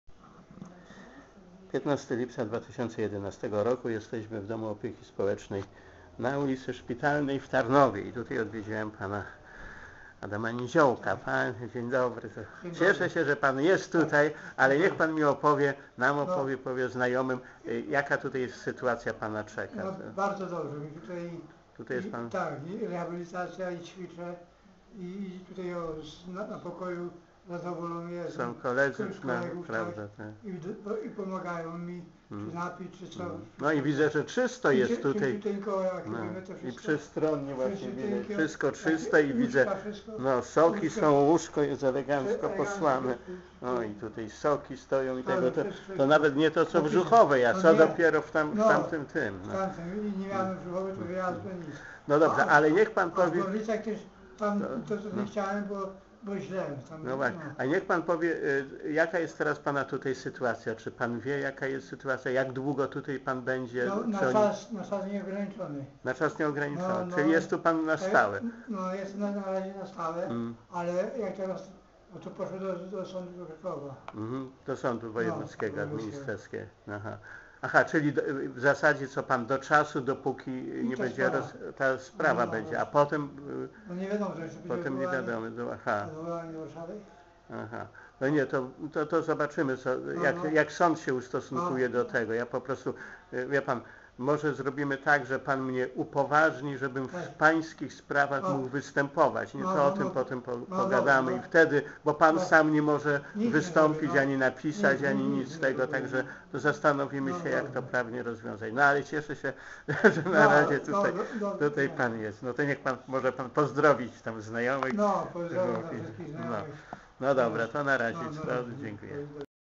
Sprawa Adama cz.8
Pan Adam 'wylądował' w końcu w Domu Opieki na ul. Szpitalnej w Tarnowie. Warunki są o wiele lepsze niż w Rzuchowej, czysto i przestronnie. Adam ma możliwość wyjechania nawet Biedronki na zakupy. Niestety z pism które mi pokazał wynika, że w Tarnowie ma zapewniony pobyt do czasu rozpatrzenia odwołania przez Wojewódzki Sąd Administracyjny.  Odwołanie, które napisano mu w Rzuchowej nie wspomina nic o tym, że Adam chce w większej części partycypować w kosztach za opiekę w Tarnowie.  O tym, w dalszych odcinkach, bo jak widać Wójt Gminy Szerzyny nie stanął na wysokości zadania.